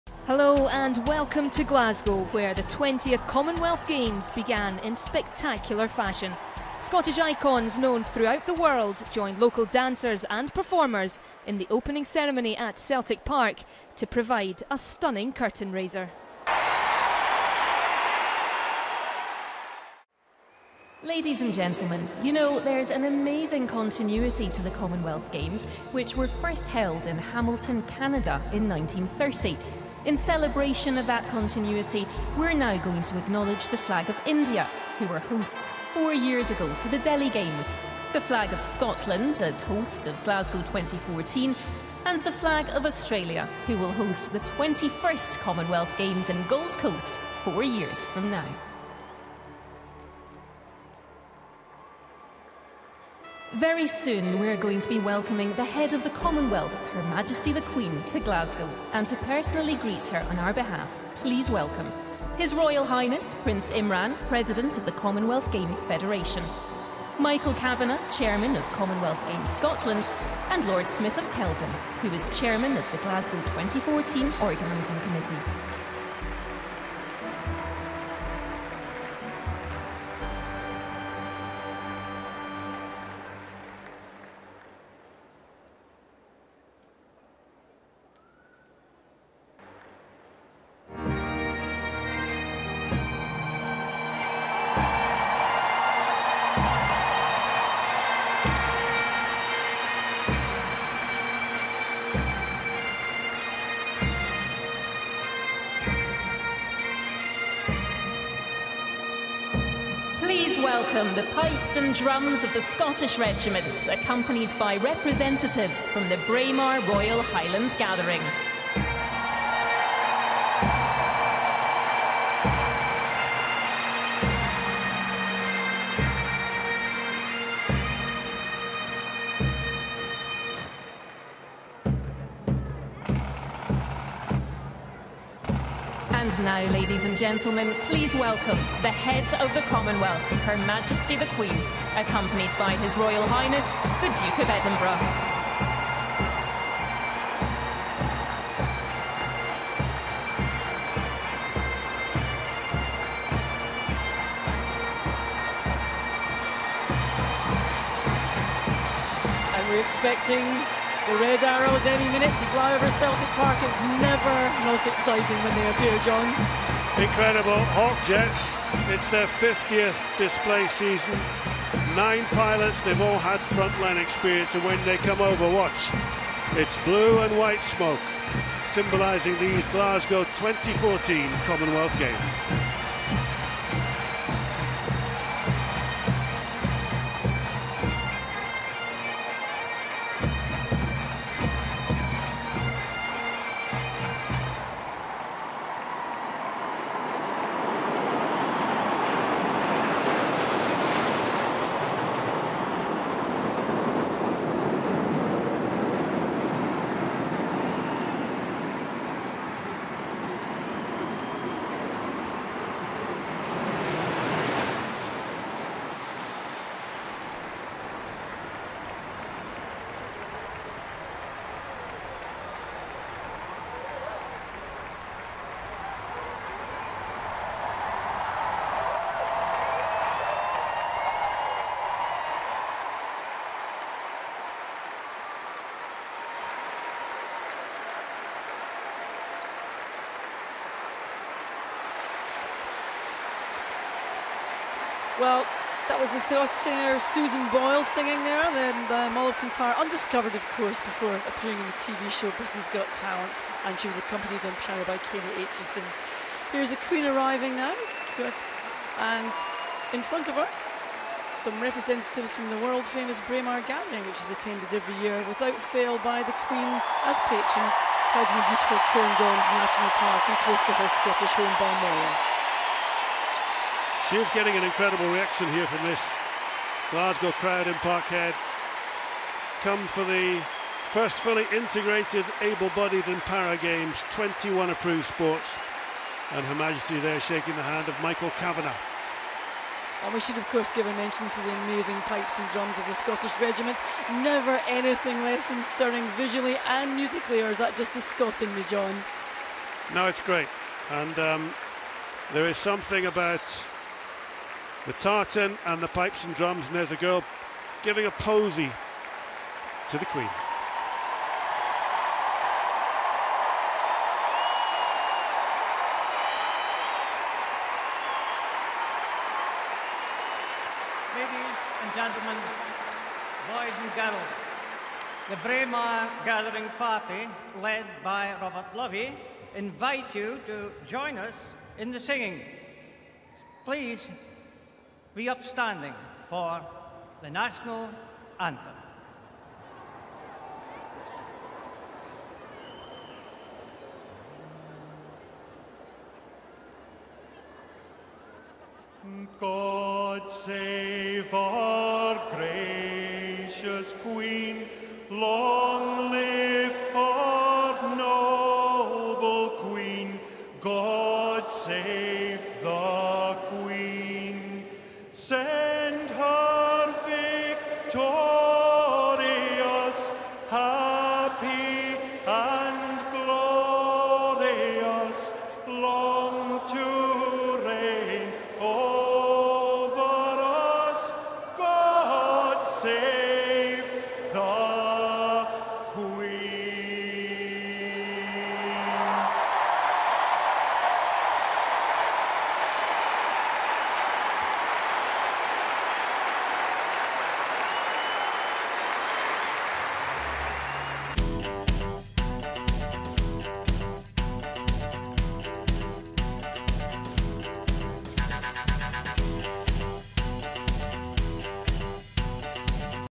Commonwealth Games Opening Ceremony - Part 1 | Glasgow 2014 Highlights
Part 1 of our highlights coverage of the Glasgow 2014 Opening Ceremony as The Queen arrives and is accompanies by musical performances and a display from the Red Arrows to kick off the celebrations of the Commonwealth Games.